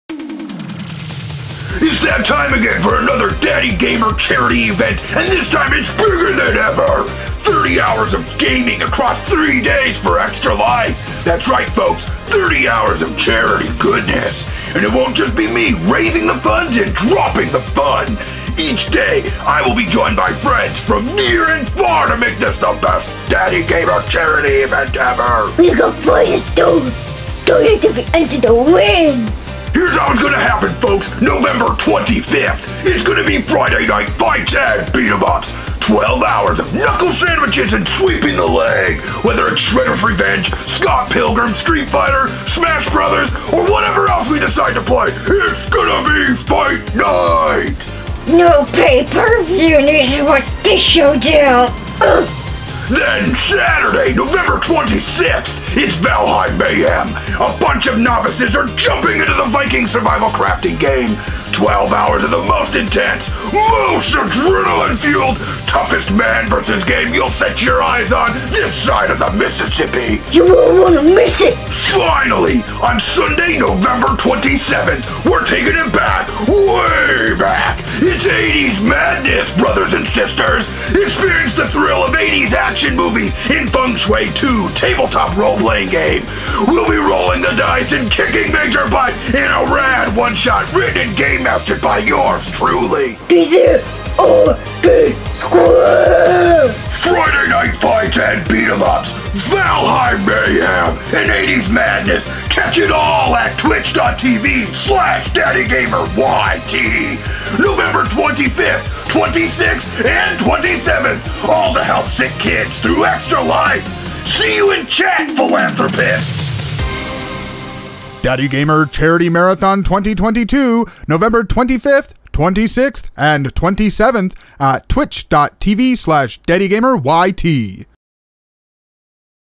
Charity 2022 Ad for extralife
Bigger and Dadder than ever before this year's Charity event is going to rock! 30 hours of streaming over 3 days all in support of Extra Life!

We got merch! 50% of profits go towards reaching our goal!